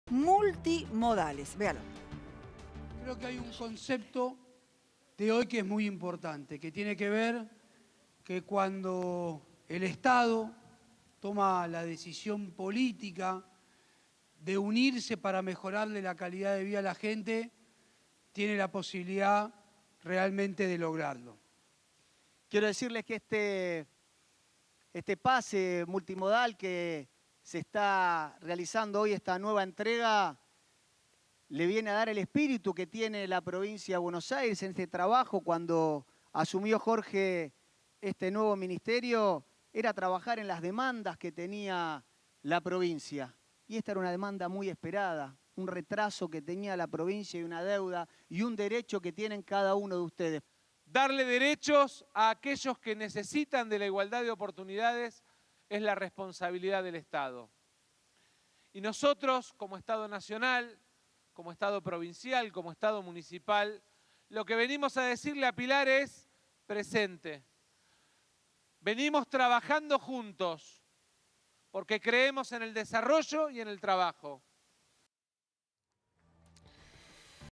Entregaron pases libres y multimodales para vecinos
Los entregó en Pilar el intendente Federico Achával junto al jefe de gabinete bonaerense Martín Insaurralde y el presidente de la Cámara de Diputados Sergio Massa.